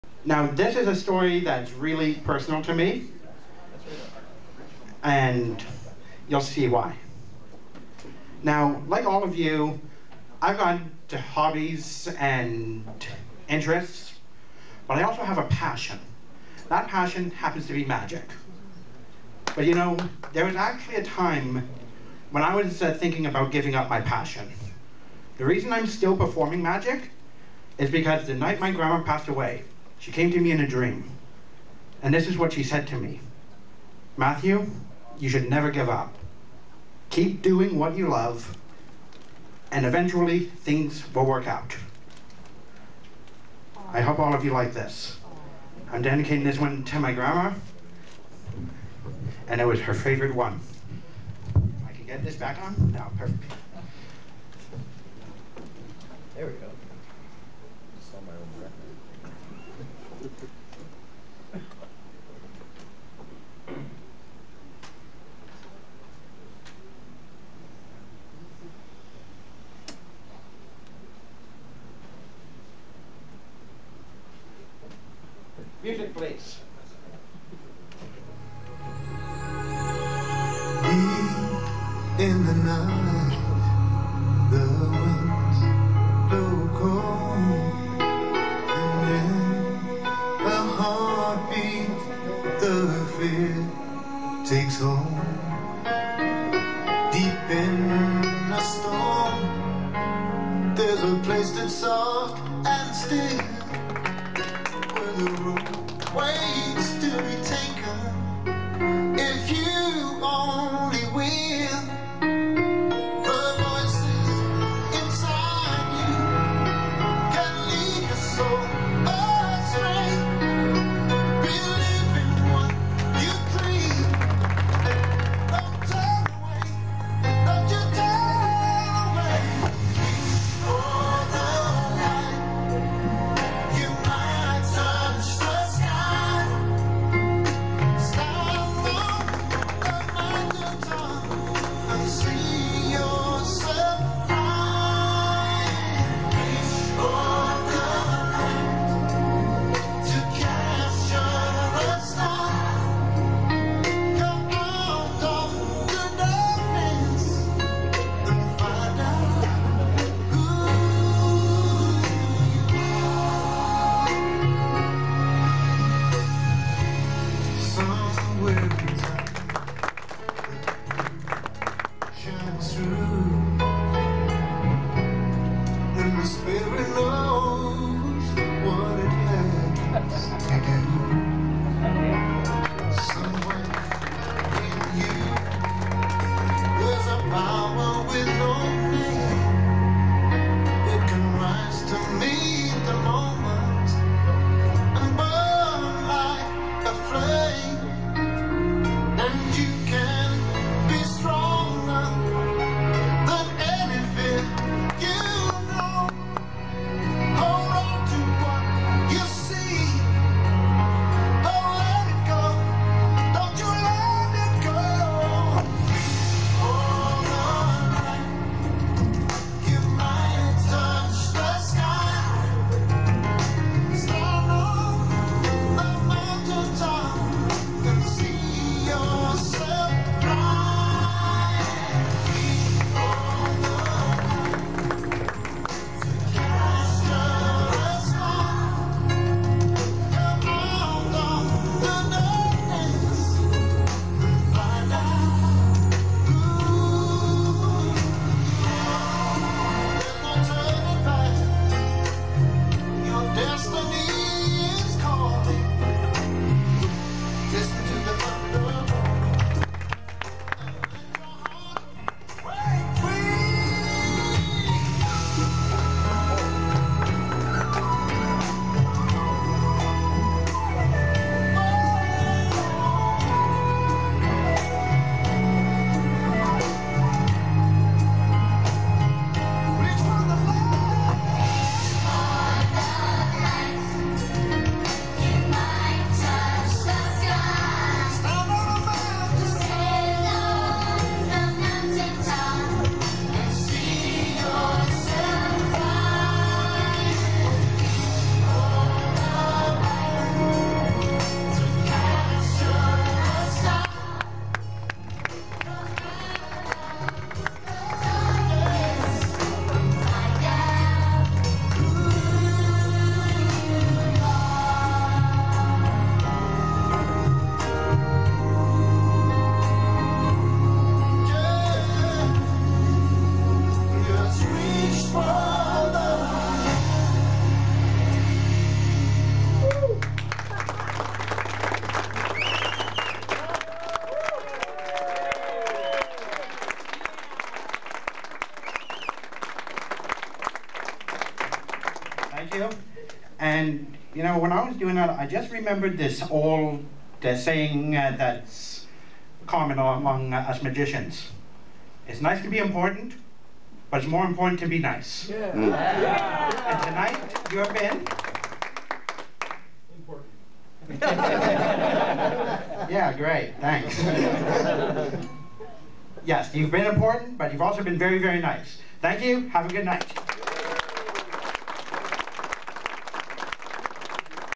Rope Routine at Three Of Clubs Convention
This is a Rope Routine I performed at a Magic Convention in Victoria, BC, Canada on November 3rd, 2012.  It took almost 5 years to get it from the beginnings to what it is today.  I originally was doing it exactly like the creator, then I started looking for songs that I could use because of the speech I was saying in the routine.  Now I know the story seems far fetched and I had to have made it up because that's what a lot of magicians do when they want to say a good story, but I promise you all the things I said in that routine are completely true.  Remember to keep the art of magic alive, don't support magic revealers on here.